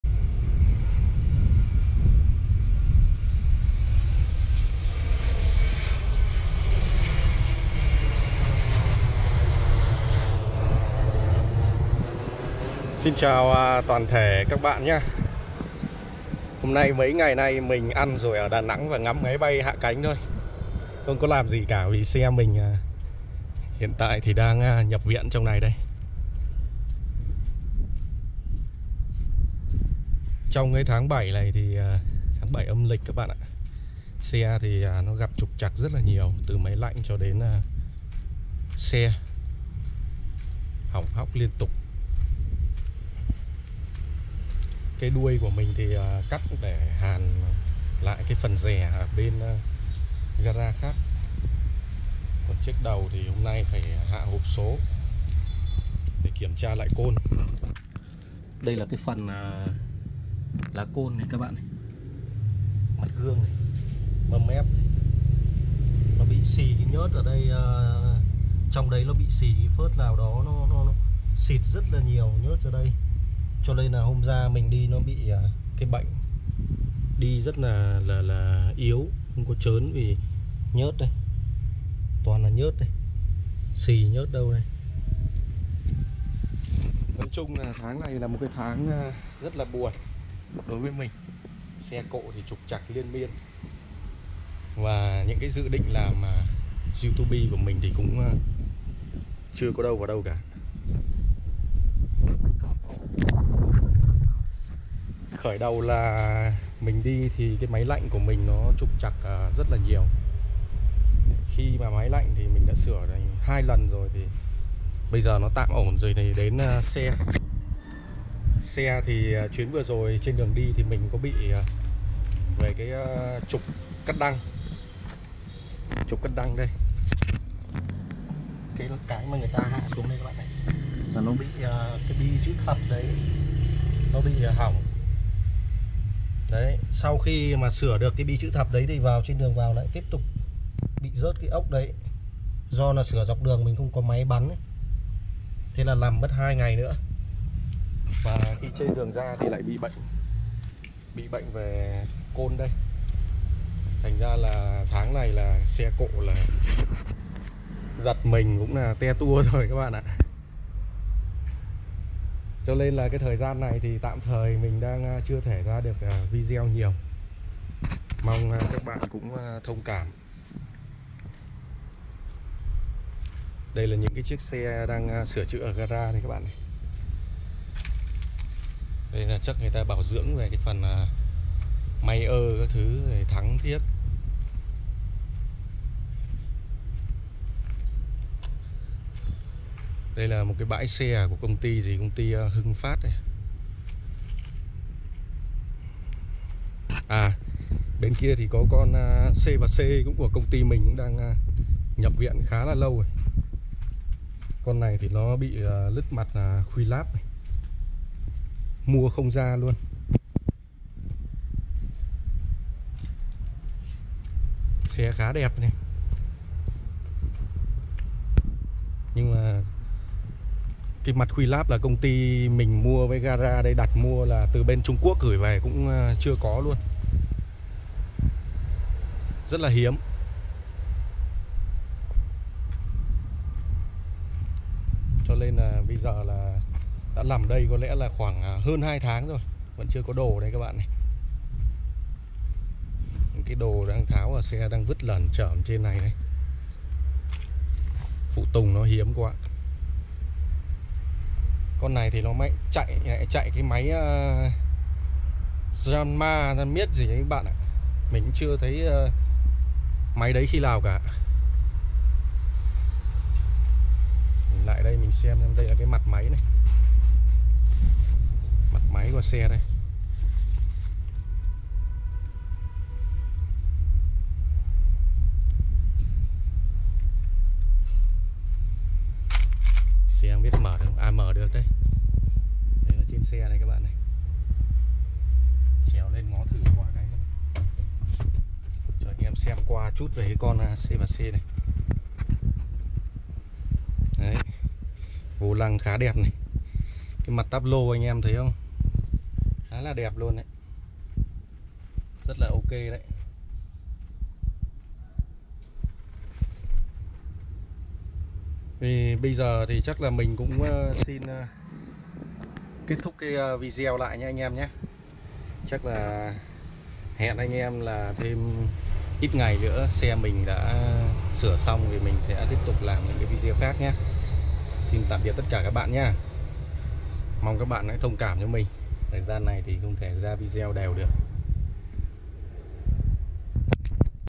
Khủng Long Nhập viện đây cũng là lý do mình ít làm video | Xe Đầu Kéo Vlog #22
Chiếc xe đầu kéo mỹ 15 năm tuổi của mình hiện tại đang nhập viên để sửa chữa , tg này mình không thể làm video nhiều được mong các bạn thông cảm.